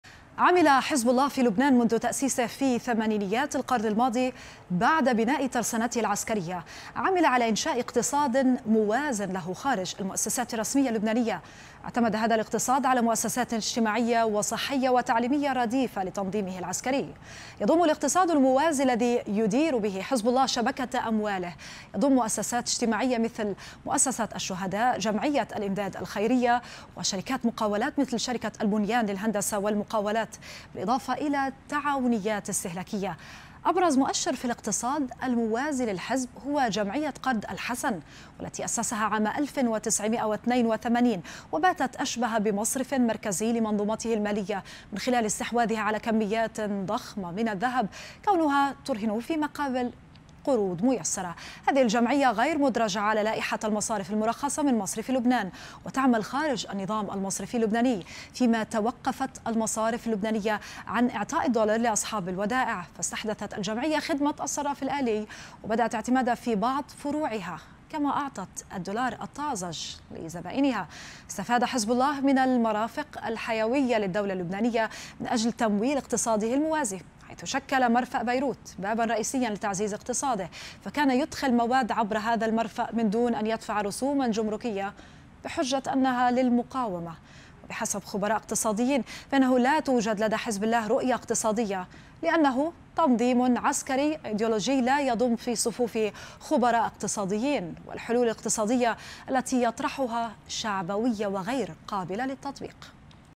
تعرف على شبكة أموال حزب الله للاقتصاد الموازي للدولة في لبنان
أبرزها "مؤسسة الشهداء" و"جمعية الإمداد الخيرية" وشركات مقاولات مثل شركة "البنيان للهندسة والمقاولات".. تعرف على شبكة أموال حزب الله للاقتصاد الموازي للدولة في لبنان